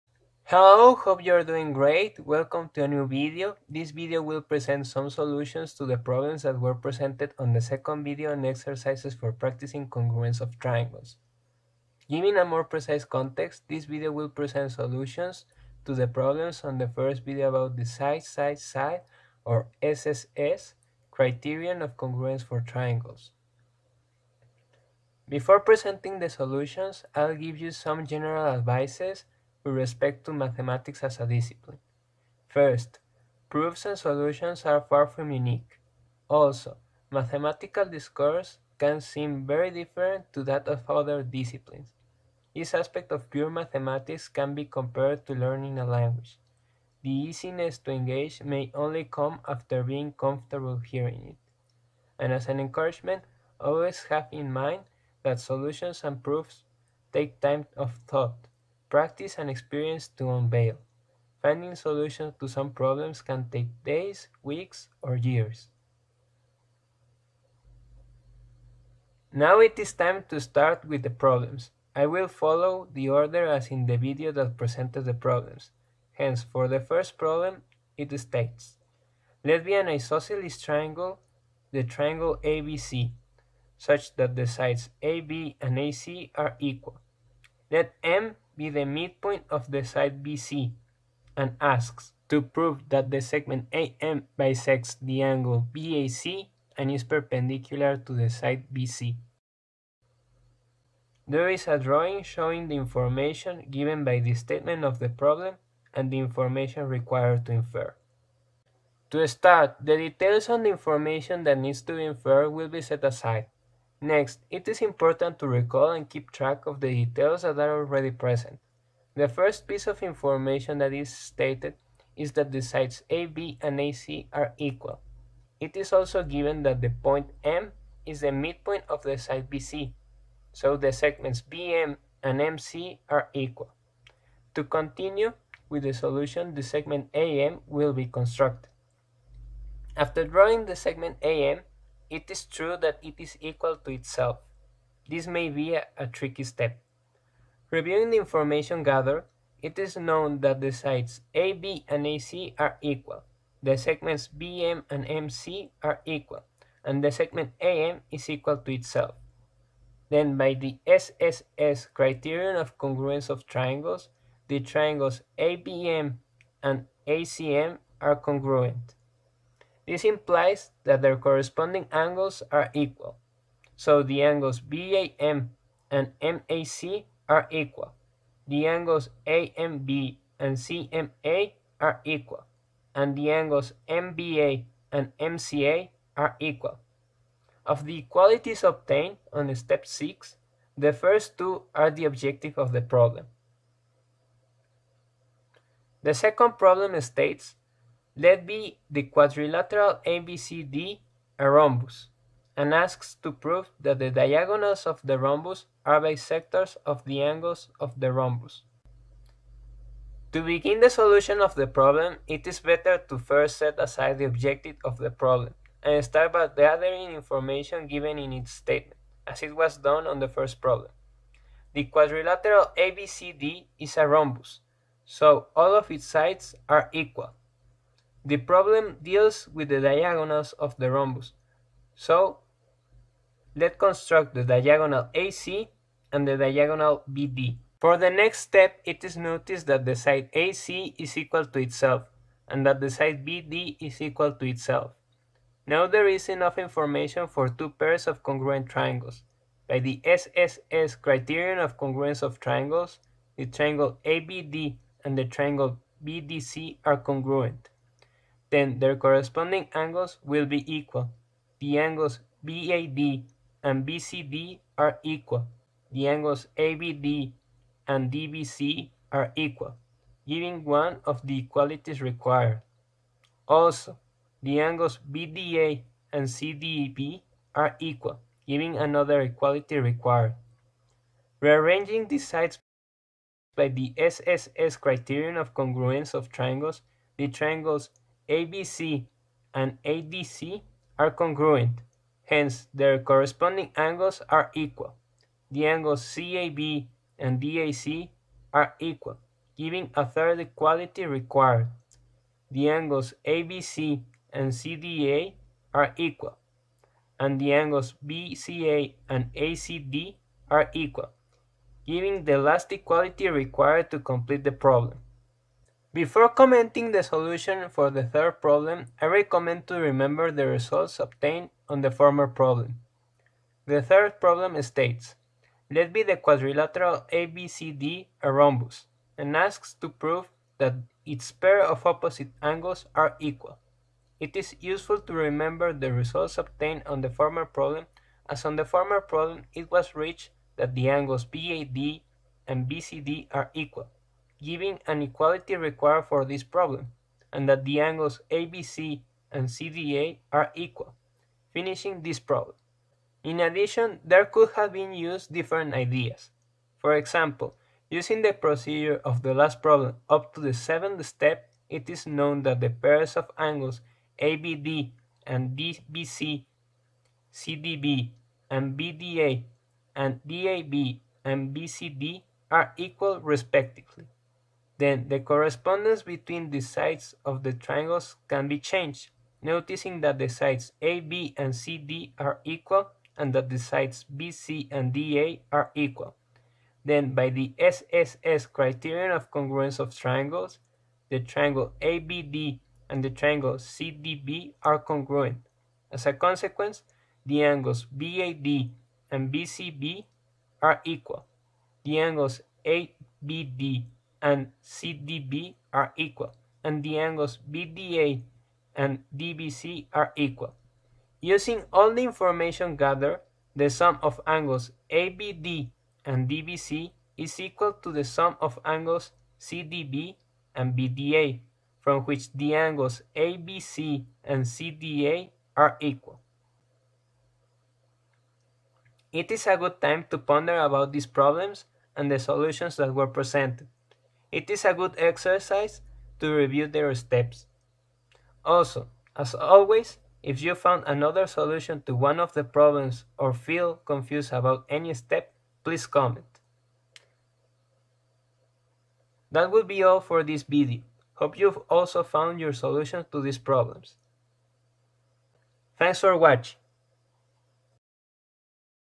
Solutions of exercises for congruence of triangles II
This video presents solutions to the problems presented on the video "Exercises for practicing congruence of triangles II".